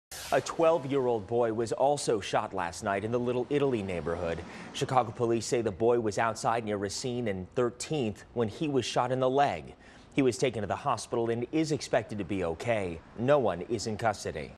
12-year-old boy shot near Little Italy sidewalk, police say
A 12-year-old boy was shot on the city's West Side, Chicago police said. FULL STORY